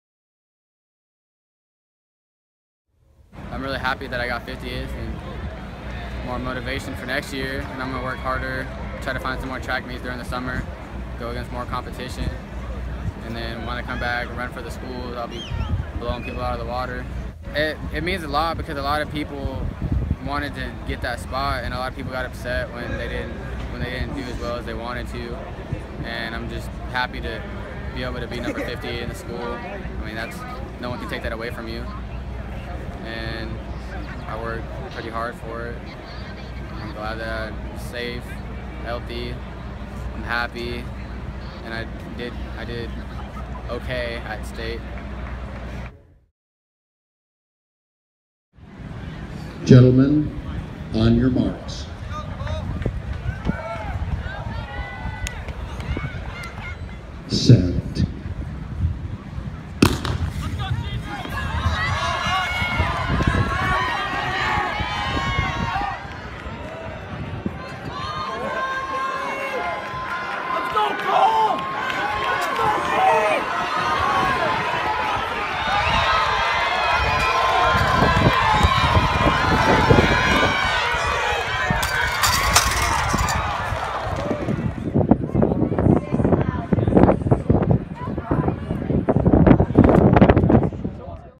Josh Braverman is River Ridge's 50th state champion
Braverman, a junior, won the 2A boys 300-meter hurdles (38.39 seconds) on Saturday, May 28, 2016 at Mount Tahoma High School to become River Ridge's 50th track and field state champion. He also took second in the 110 hurdles (14.48), fifth in the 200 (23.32) and sixth in the long jump (21 feet, 7 inches) at Star Track XXXIV.